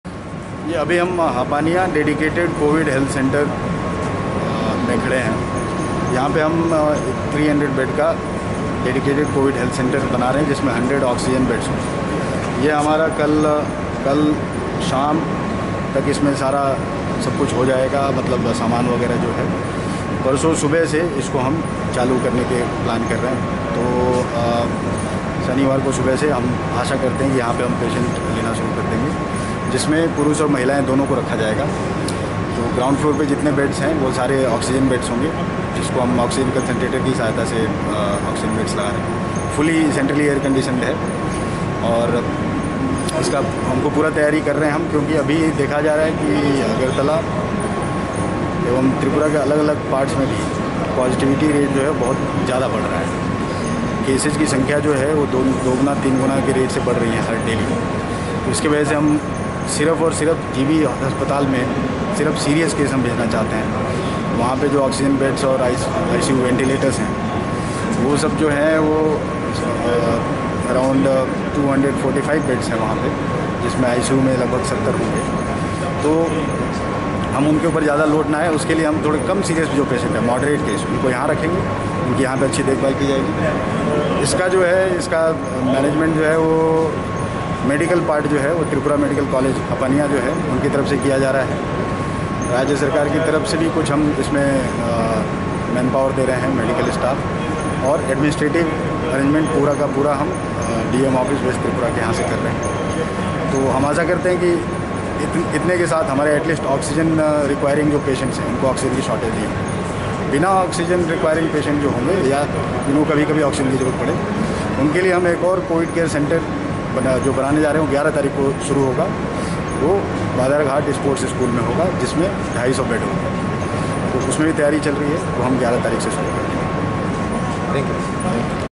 Covid Cases sparks high : Hapania COVID Centre will be operational from Saturday.
Covid Cases sparks high : Hapania COVID Health Centre will be operational from Saturday. DM West checked Arrangements.